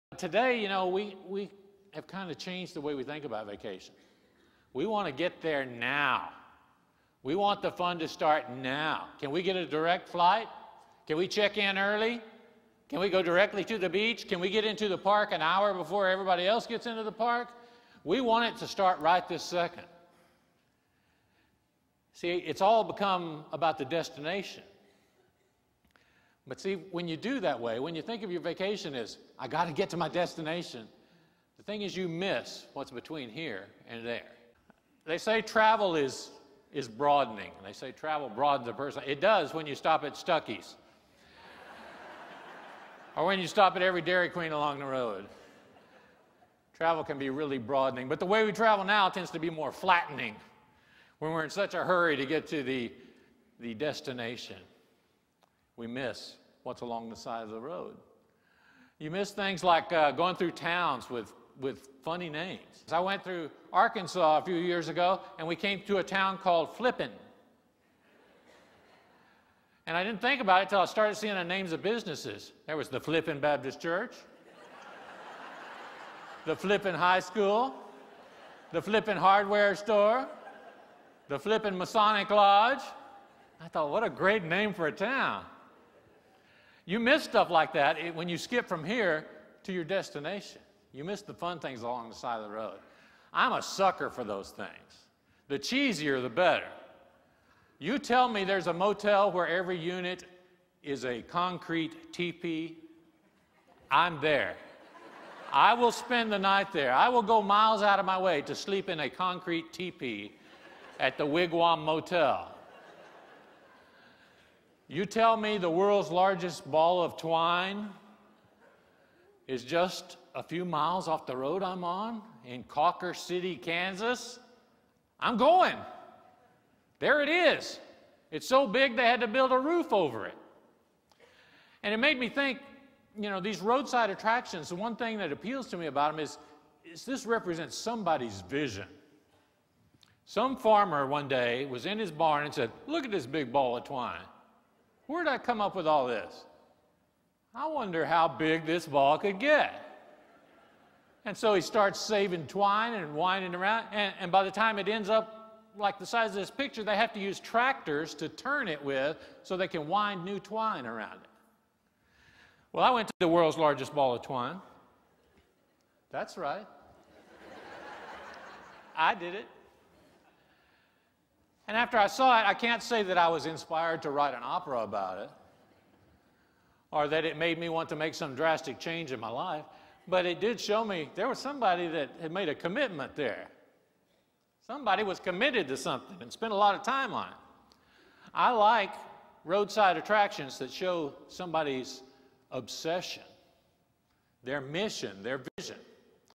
Mike Robertson: sample of speaking style
Mike Robertson is an Austin-based speaker, writer and raconteur, whose unique style combines genuine humor with unusual insights and truly inspiring stories. and get this Mike on your stage.